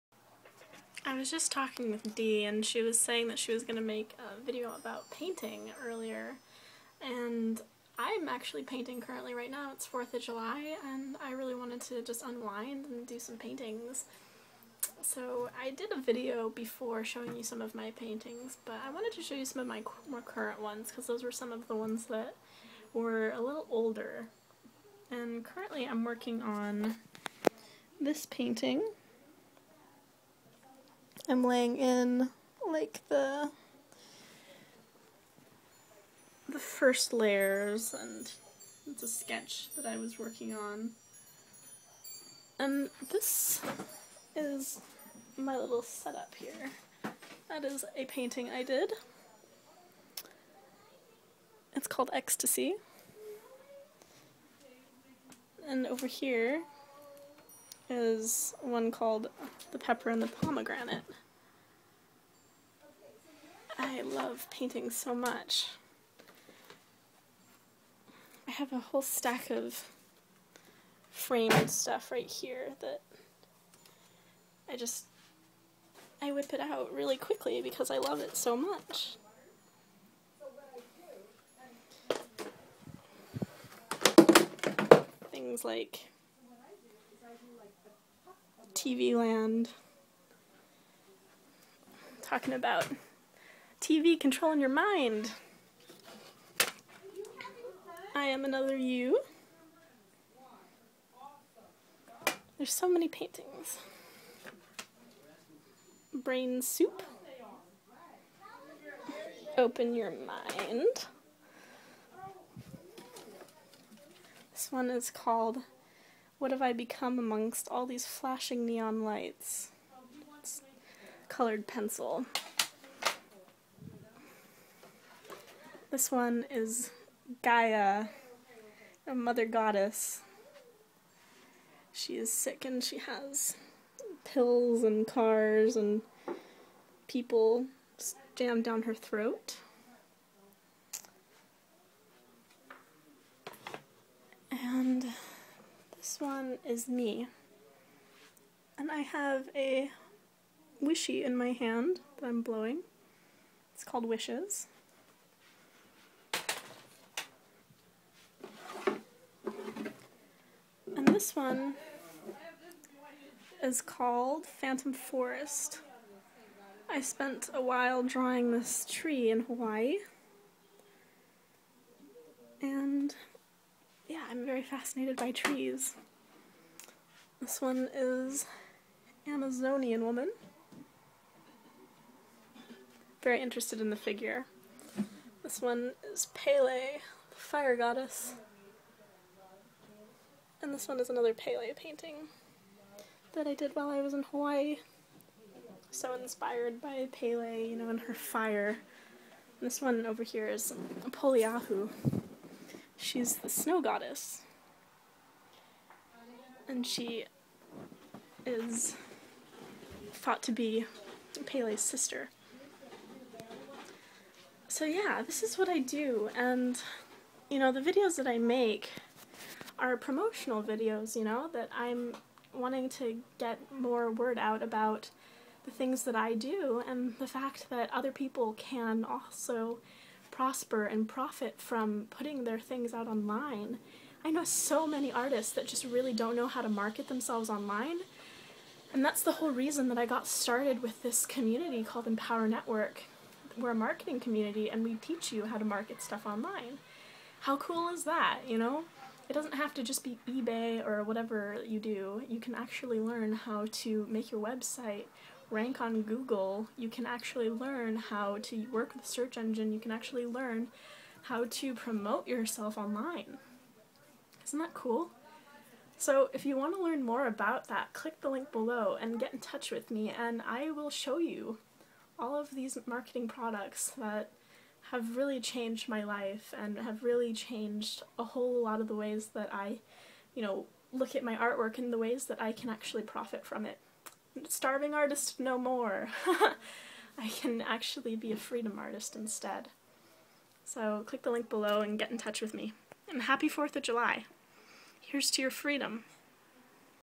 A Tour Of My Art Space (Artist Katerina Edwards)
Tour Of My Art Space! Paintings, current works, etc.